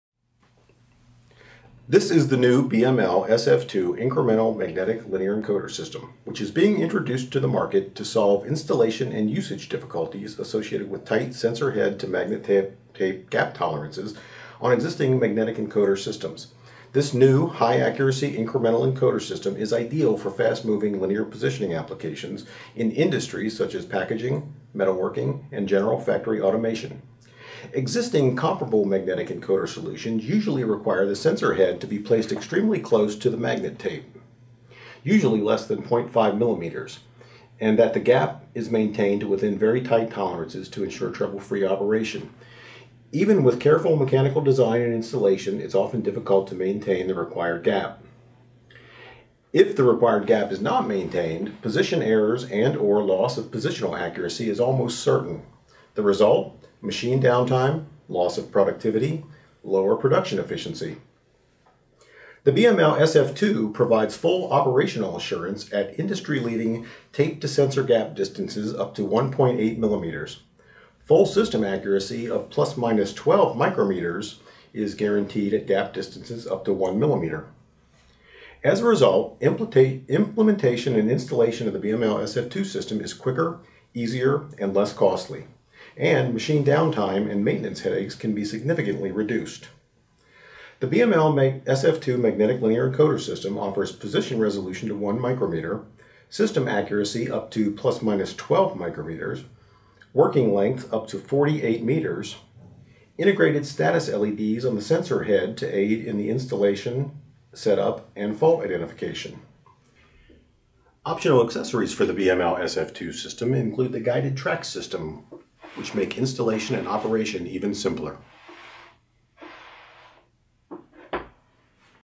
Magnetic Linear Encoder with Wide Gap Tolerance
A brief overview of the new BML SF2 incremental linear encoder system.  The BML SF2 offers unprecedented sensor-to-magnet tape gap tolerances which ensures easier, faster installation, and trouble-free performace.